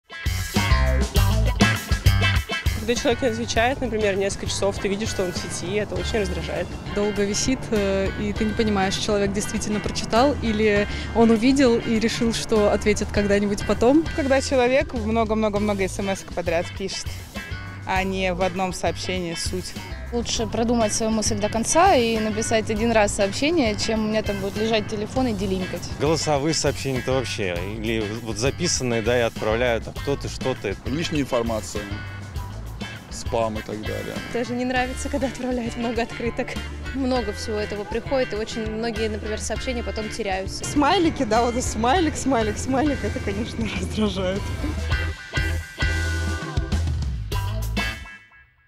Что вас раздражает при общении в мессенджерах?
Узнаем у петербуржцев, какая форма общения им не по душе. Опрос на улицах города